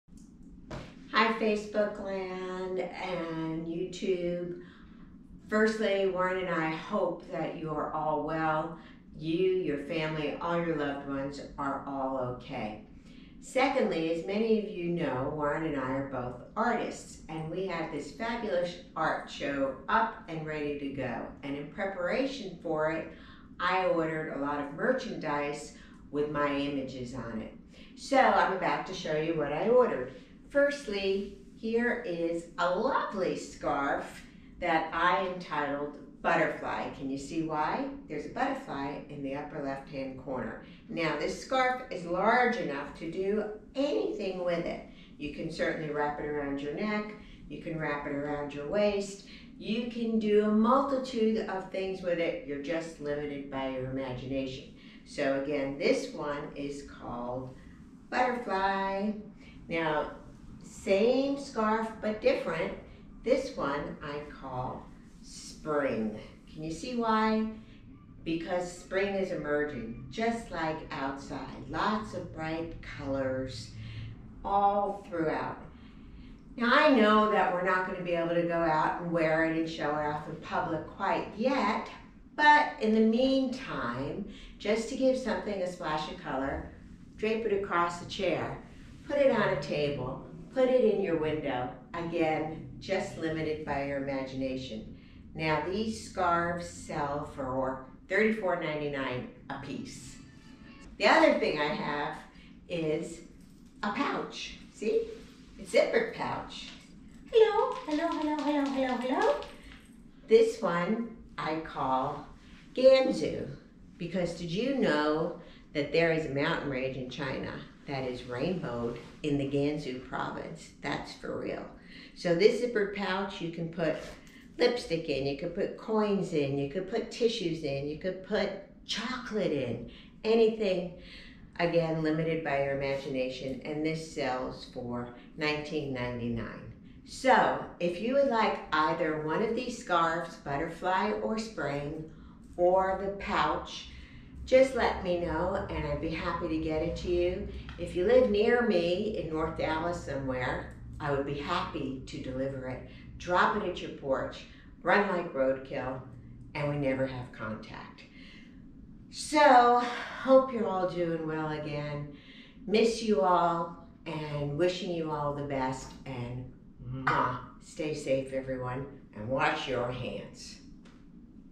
Abstract Art Scarves and Zipper Pouches
Karen Harris showing two of her abstract art scarves and another design in a small zipper pouch, perfect for cosmetics or...

Order all my work at hhtps://karenrharris.redbubble.com